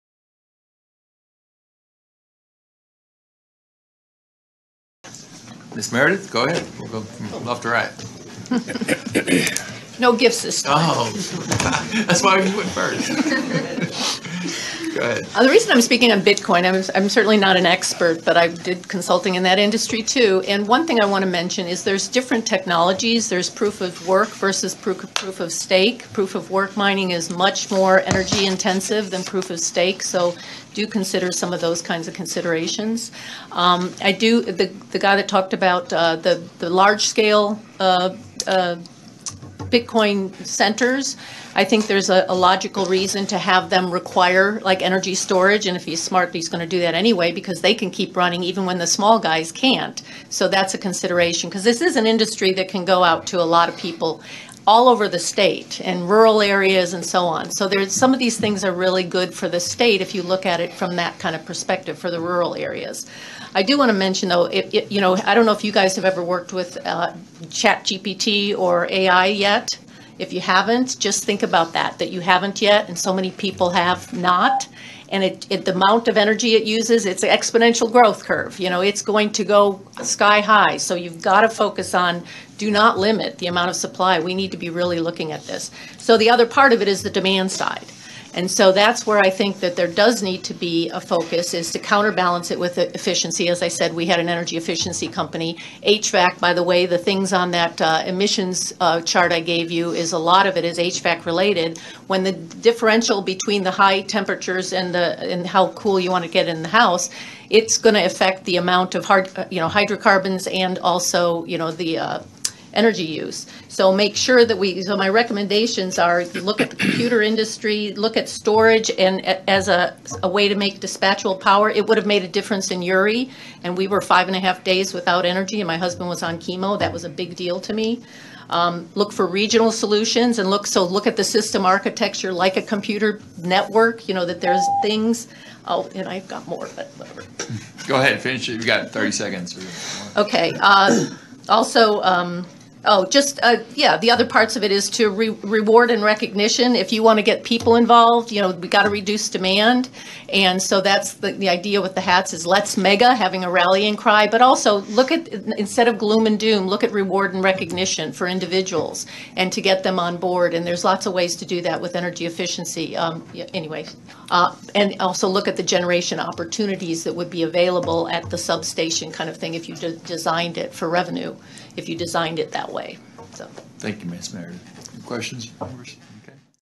2024-06-12 Testimony before the Texas Senate Business & Commerce Committee-BCM (Susan Meredith)
A big thank you to Austin area CCL volunteer Susan Meredith for patiently listening to 7 hours of invited testimony at the June 12, 2024, hearing of the Texas Senate Business & Commerce Committee before being invited to provide her comments during the public testimony section of the hearing on Impact of Bitcoin Mining on the Texas Electric Grid.